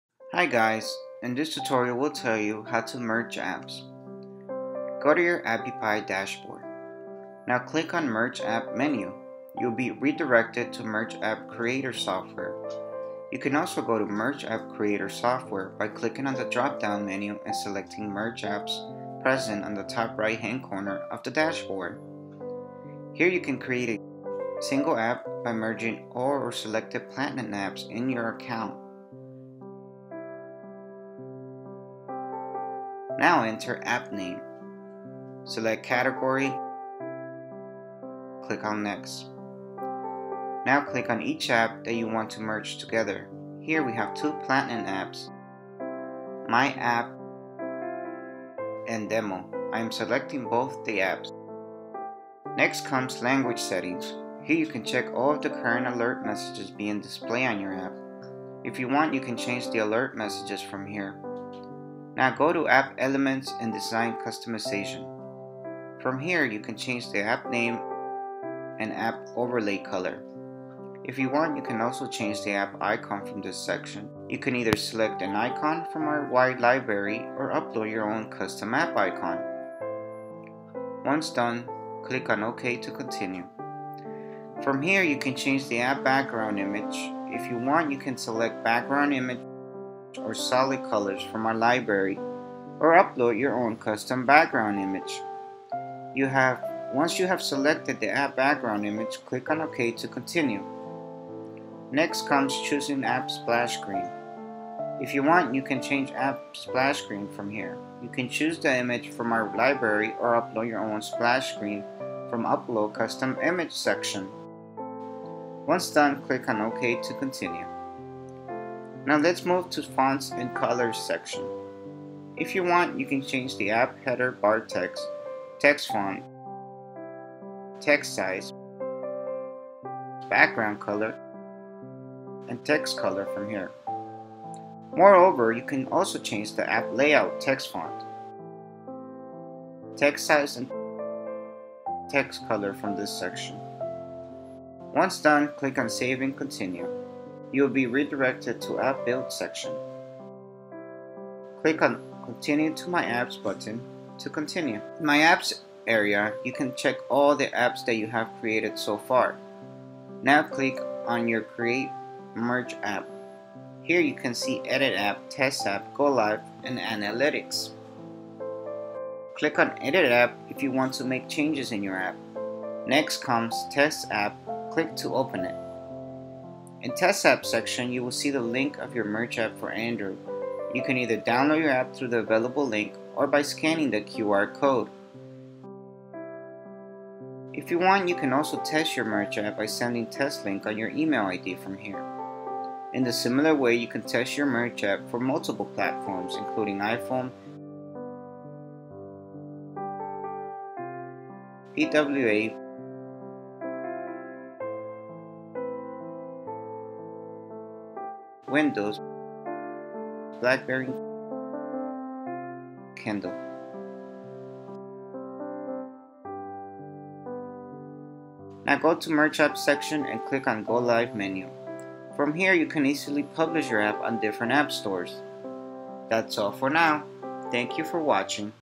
How to Merge Apps on Appy Pie?
Learn how to create an app for Android or iPhone with App Builder Appy Pie in three simple steps, and merge all your Platinum apps without any coding skills.